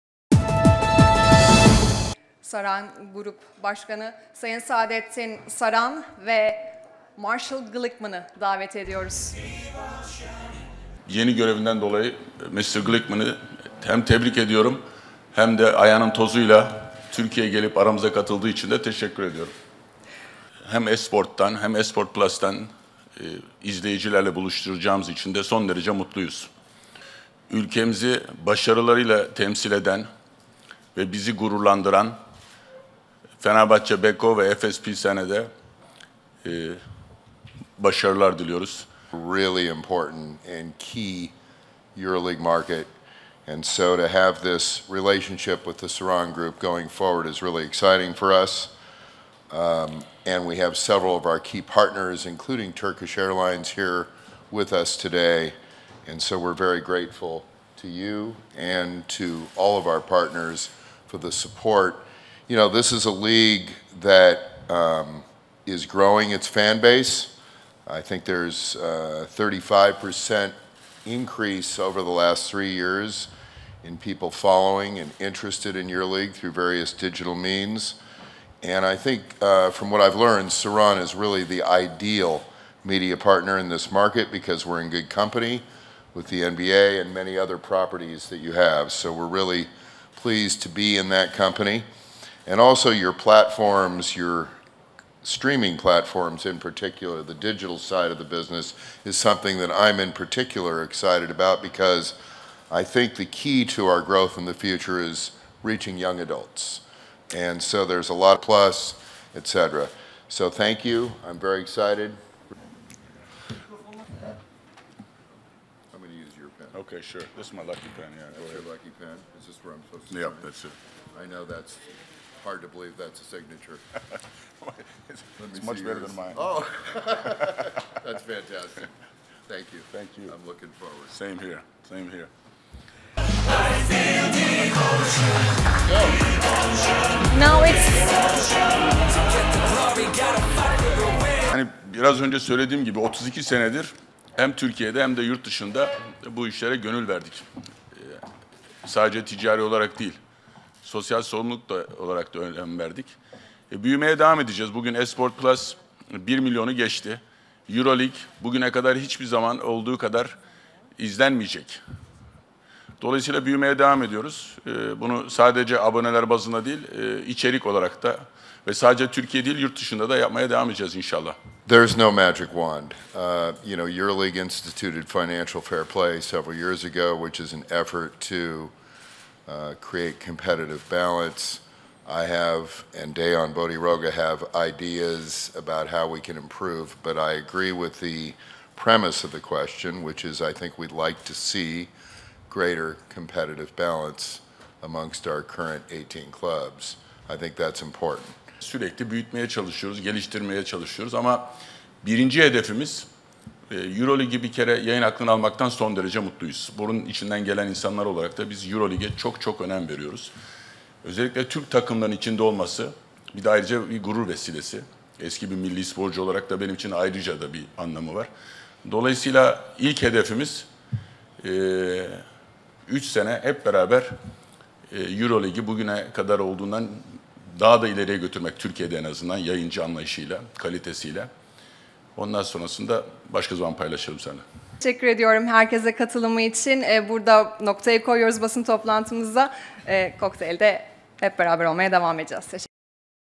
THY Avrupa Ligi Maçları S Sport ve S Sport Plus'ta Yayımlanacak
Basketbolda THY Avrupa Ligi maçları 3 sezon boyunca S Sport ve S Sport Plus'ta yayımlanacak.
Beyoğlu Soho House'da düzenlenen basın toplantısına Saran Group Yönetim Kurulu Başkanı Sadettin Saran ile Avrupa Ligi Başkanı Dejan Bodiroga ve Avrupa Ligi CEO'su Marshall Glickman katıldı.
Burada bir konuşma yapan Sadettin Saran, Marshall Glikcman'ı yeni görevinden dolayı tebrik etti.
Saran Group'un 32 yıldır Türk sporuna katkıda bulunmaya çalıştığını ve Basketbol Avrupa Ligi'ni iki kanaldan izleyicilerle buluşturdukları için son derece mutlu olduklarını aktaran Saran, "Ülkemizi başarılarıyla temsil eden ve bizi gururlandıran Fenerbahçe Beko ve Anadolu Efes'e de başarılar diliyoruz. İnşallah minimum 3 senemiz çok iyi geçer." dedi.
32 senedir Türkiye ve yurtdışında spor işlerine gönül verdiklerini, sadece ticari değil sosyal sorumluluk bilinciyle de hareket ettiklerini vurgulayan Saran, "S Sport ve S Sport Plus abonelik sayısında bir milyonu geçti.
Saran, sürekli gelişmeye çalıştıklarını anlatarak, "Birinci hedefimiz Avrupa Ligi hakkını almaktı. Gerçekleştirdiğimiz için çok mutluyuz, buna çok önem veriyoruz. Ligde Türk takımları olması da gurur verici. İlk hedefimiz, Avrupa Ligi'ni yayın anlayışında 3 sene hep beraber bugün olduğundan daha ileriye götürmek. Sonraki hedeflerimi de başka zaman paylaşırım." değerlendirmesinde bulundu.